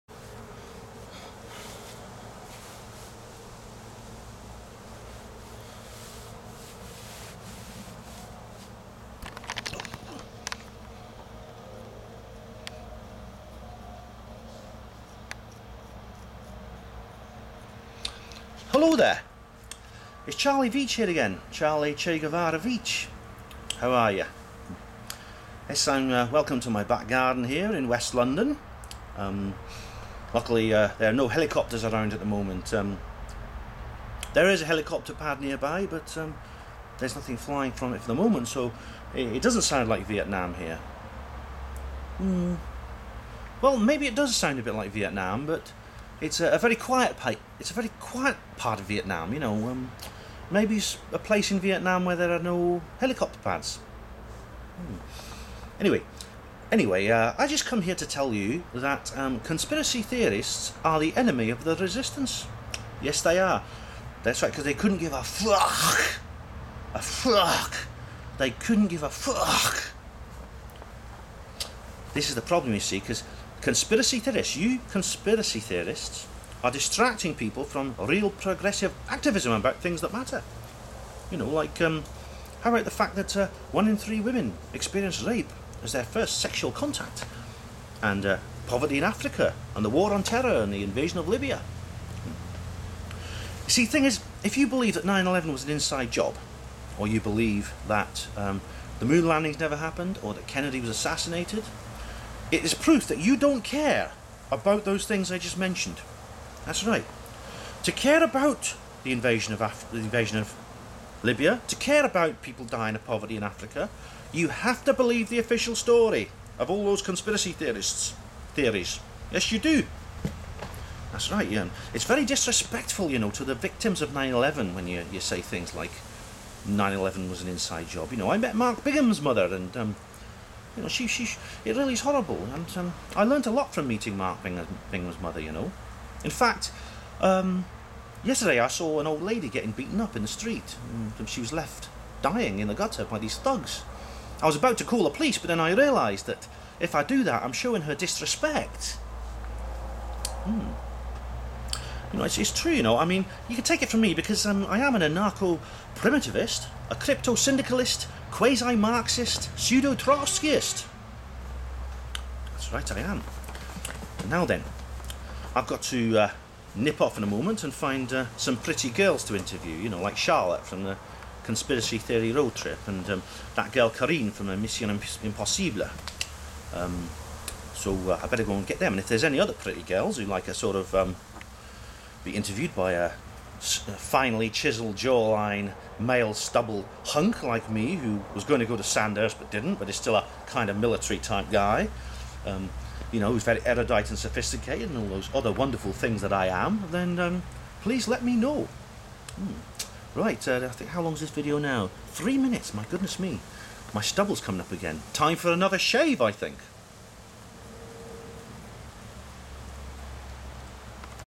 Che Gue-Veitch explains why Conspiracy Theorists couldn't give a Faaahhuck!
Hello, it's Charlie Che Guevara Veitch here again in my back garden. I'll tell you why Conspiracy Theorists are the enemies of the Resistance.